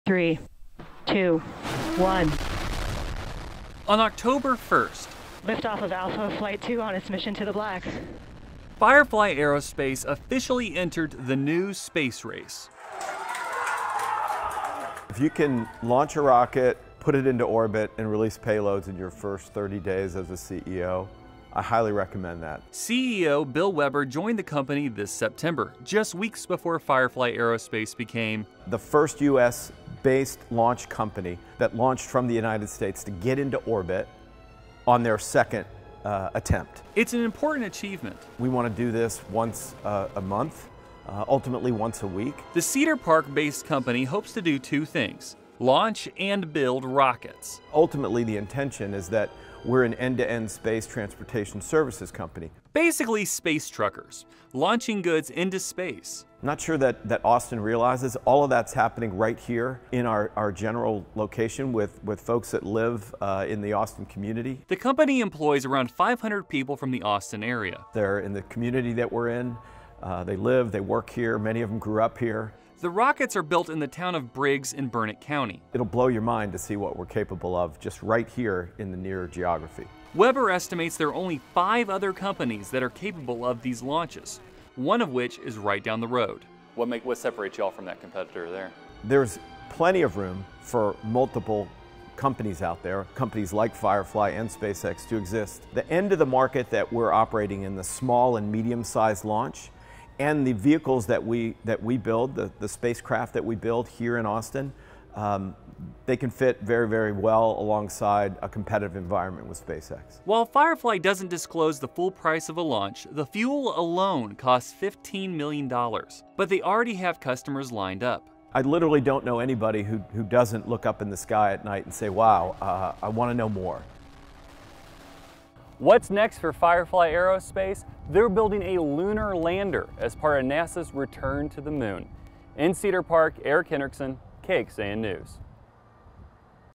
‘Secret’ Austin-area aerospace company breaks space flight record
Another one of Central Texas’ “best-kept secrets,” Firefly Aerospace, is breaking new ground when it comes to space transportation. Earlier this month, the company broke a new record, becoming “the first U.S.-based launch company that launched from the United States to get into orbit on their second attempt,” said CEO Bill Webber.